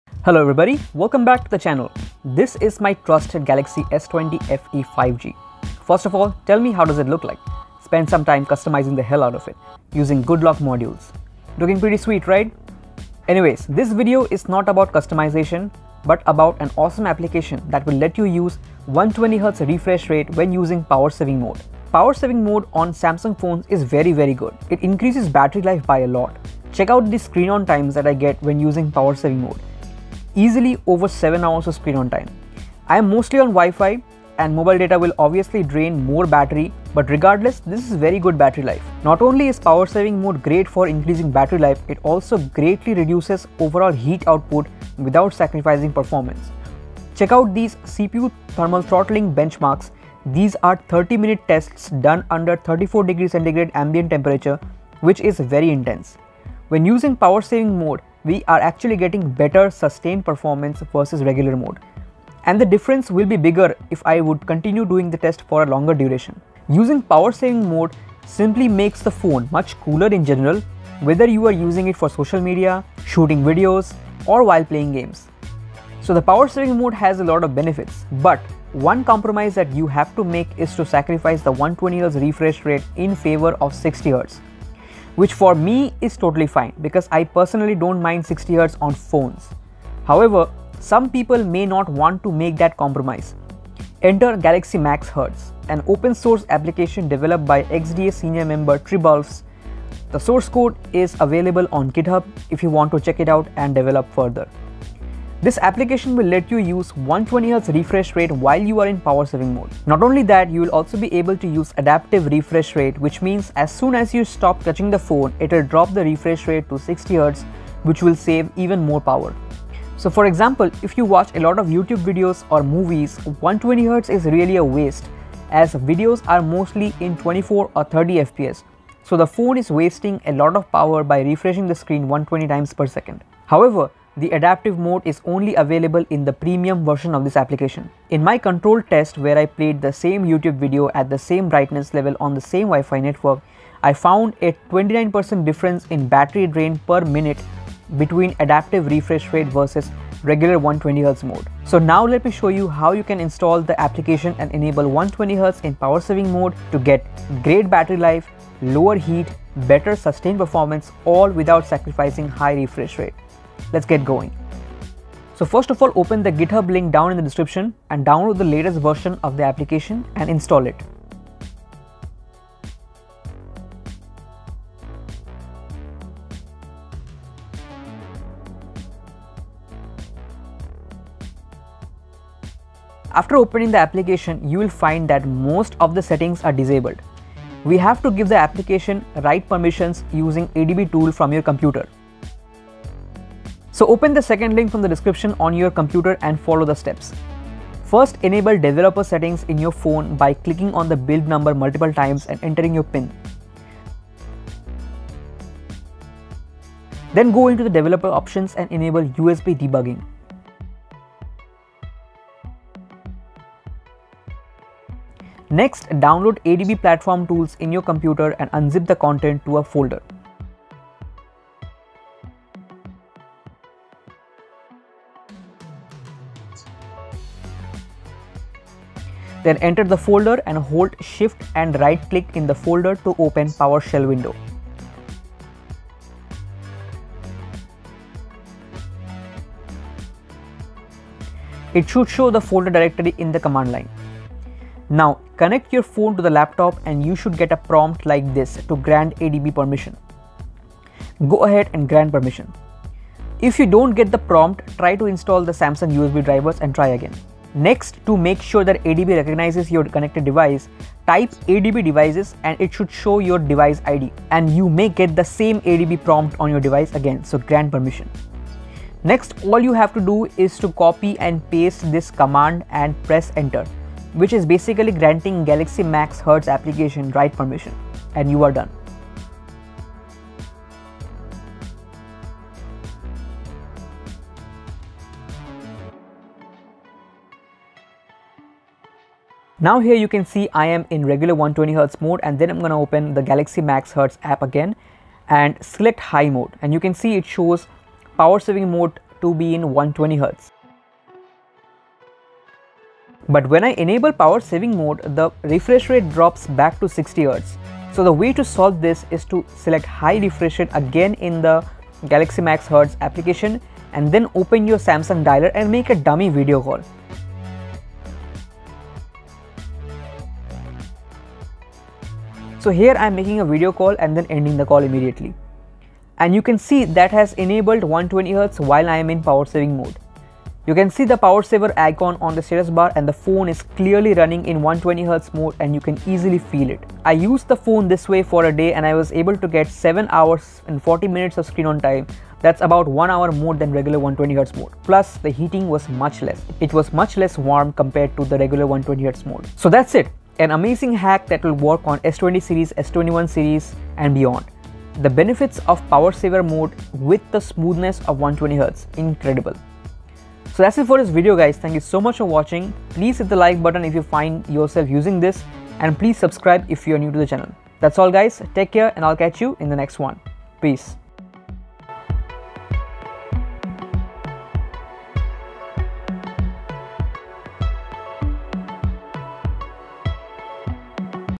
ULTIMATE GALAXY HACK | Enable 120Hz in Power Saving Mode | Less Heat Long Battery 😀S20/S21/S22
Power Saving Mode is a life saver for galaxy users! It improves battery life, lowers heat and also improves sustained performance! But you had to sacrifice the 120hz refresh rate! NOT ANYMORE!

Massive thanks to Senior XDA Dev /tribalfs 😊